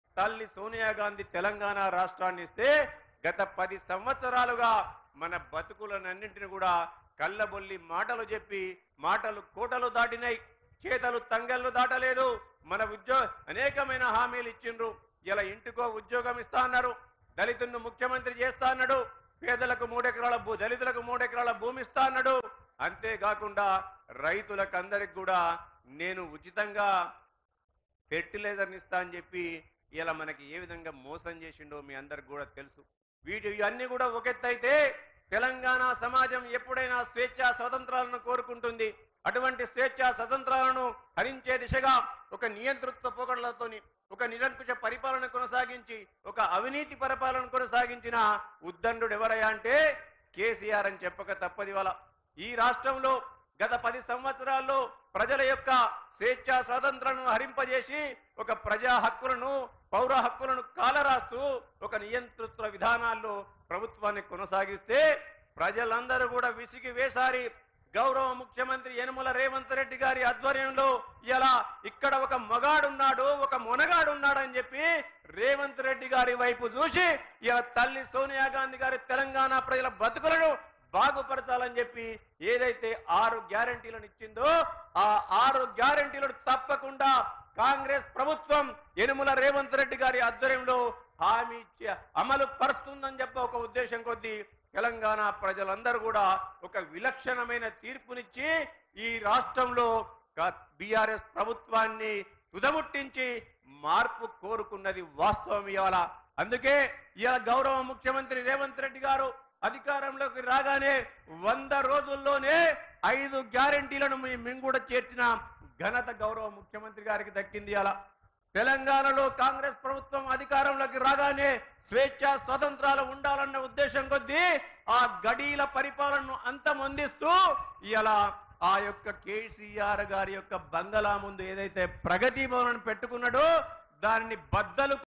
KK Mahendar Reddy About CM Revanth Reddy : రేవంత్ మగాడు .. మొనగాడు | BIG TV
CM Revanth Reddy Public Meeting At Sircilla: కేటీఆర్ అడ్డాలో  రేవంత్ మాస్ స్పీచ్ | BIG TV